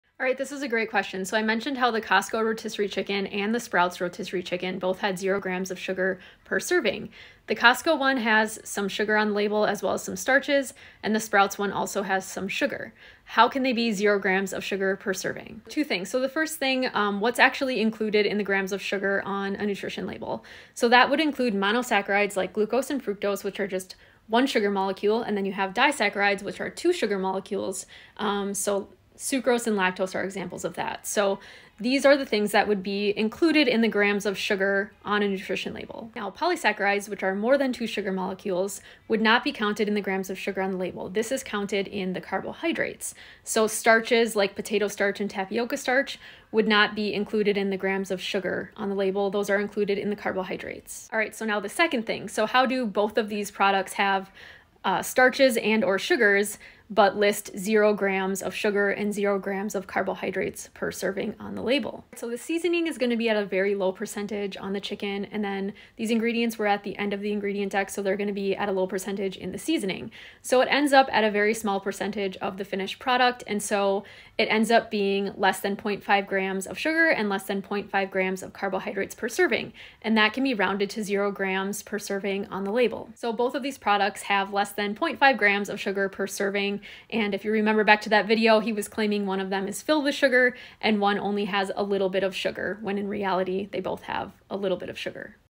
Sugars and Carbohydrates
“How does Costco chicken have 0 sugar but also lists food starch, tapioca, potato. Don't these have sugar?” Good question!

Carbohydrates include grams of sugar as well. So, carbohydrates include mono-, di-and polysaccharides, but only mono and disaccharides are counted as sugars.

Also, I meant Wegman’s, not Sprouts. Although the sprouts one contains sugar too, they just list it as “evaporated cane sugar” 😅 gotta love marketing.